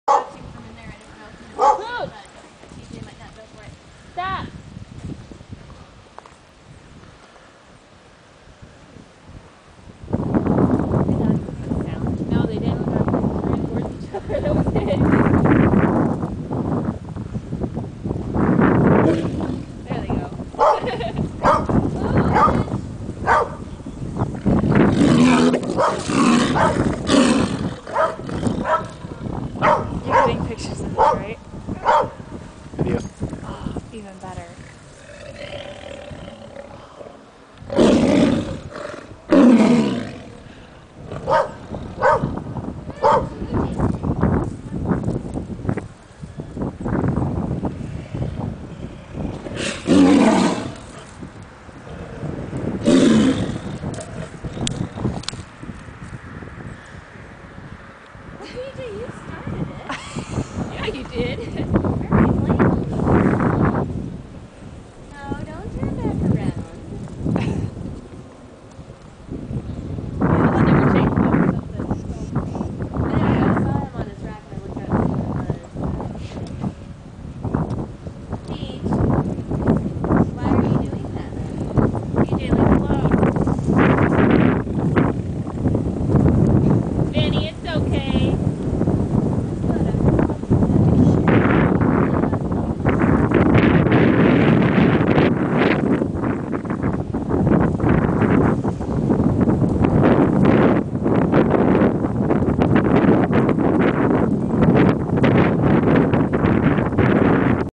T.J. and Vincent the tigers playing at the National Tiger Sanctuary!
T.J. and Vincent the tigers at the National Tiger Sanctuary in Missouri.